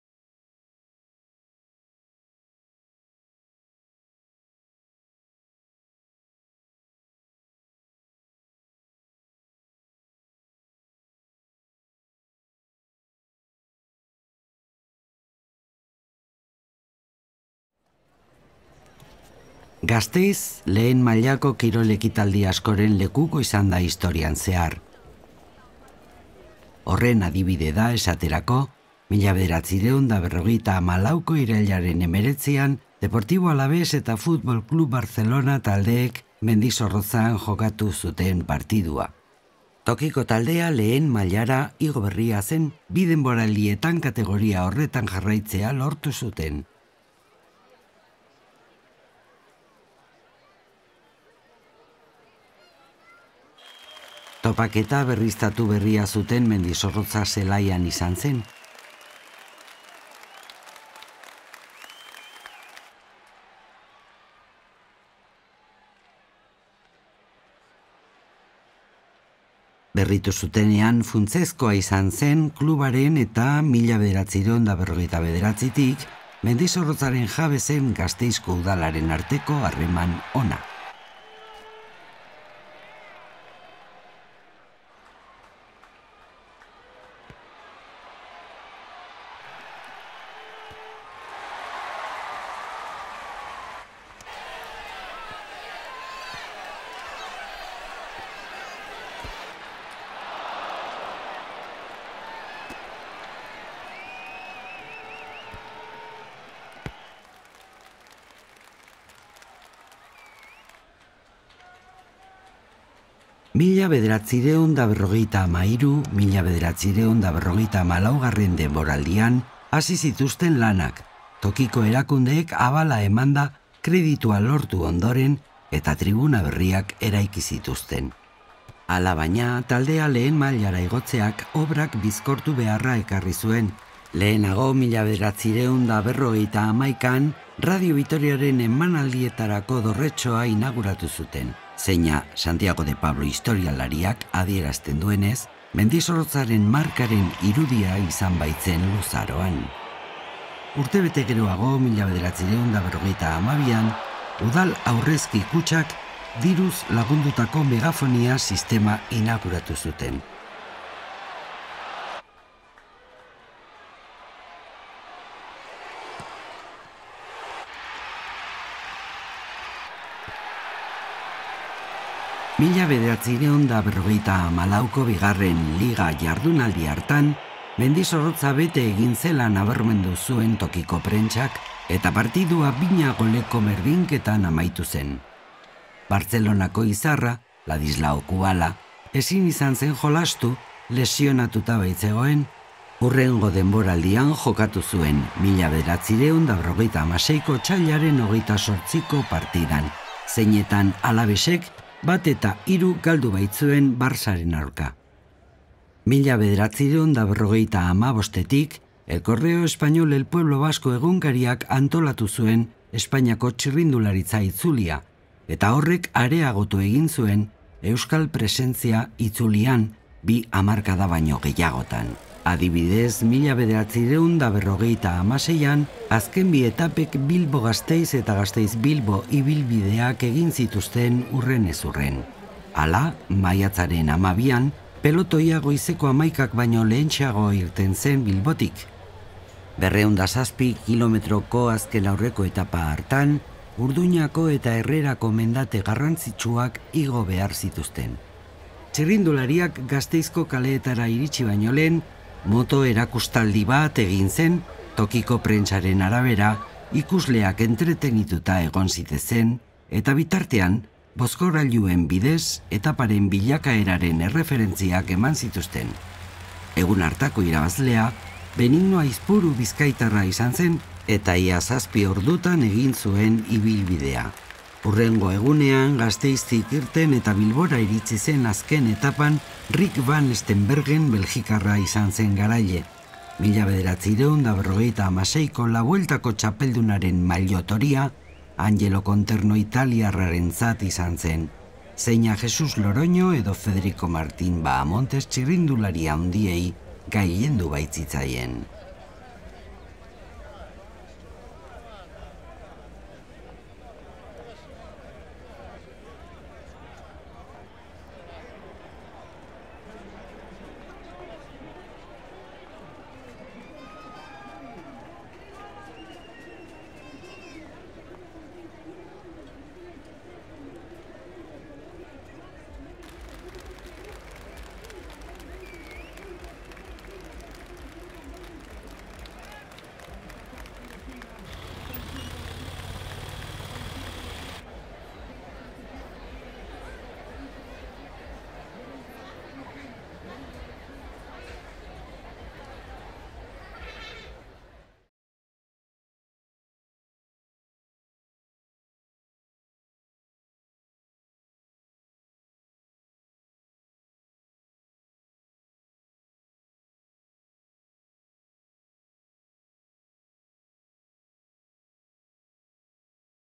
ALAVÉS-BARCELONA (1954) ETA ESPAINIAKO ITZULIA (1956)
“Benetako kolorea” deritzon egitasmoak Juan Manuel Alfaro Caballerok 1954 eta 1959 artean Gasteizen grabatutako koloretako filmak berreskuratu ditu. Balio historiko handiko dokumentuak dira, estreinakoz ikusgai. Hiriak industria eta demografia eraldaketa hasi baino lehen zuen itxura eta bizimodua erakusten dute.

Irudiak bobina hauetakoak dira:

9. zk., 1956. urtea: Espainiako Txirrindulari Itzuliaren etapa Gasteizera iristea (maiatzak 12).

7. zk., 1954. urtea: futbol partida; Deportivo Alavés - F.C. Barcelona (irailak 19).

Beste hedabide batzuetan partekatu edo txertatzeko, derrigorrezkoa da bideo honen URL helbidea estekatzea.